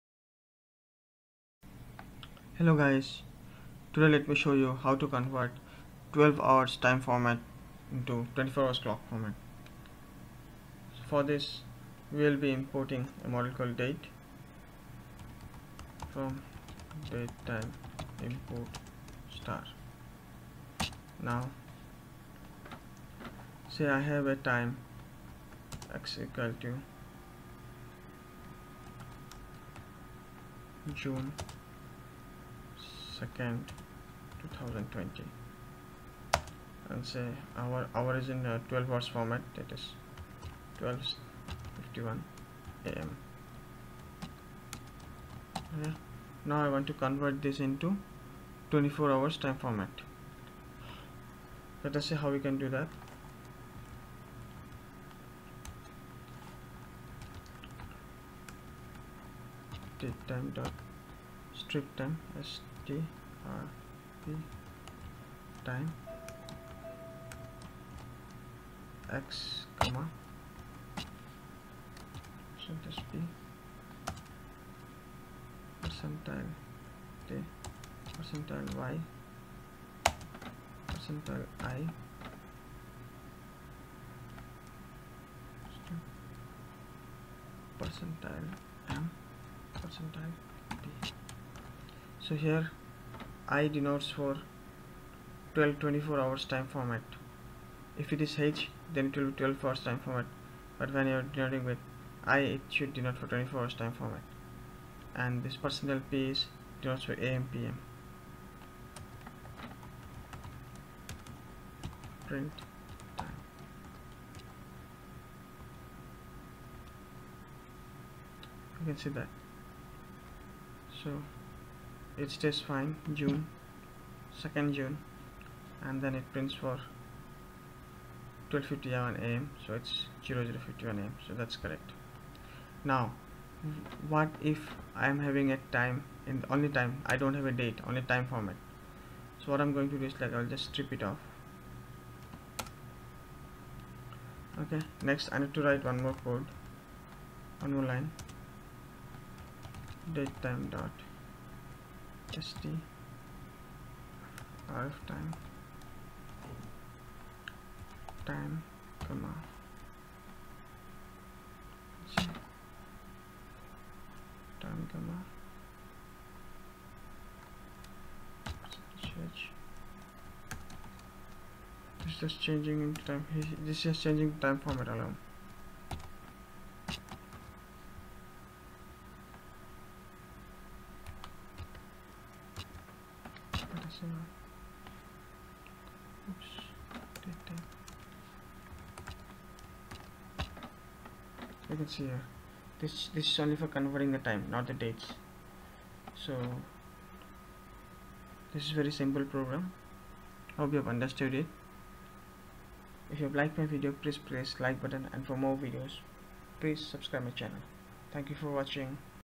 Converting 12 Hrs time format to 24 Hrs || Python datetime module
This video shows how to covert time format. Link for Python Introduction and Python interpreter installation :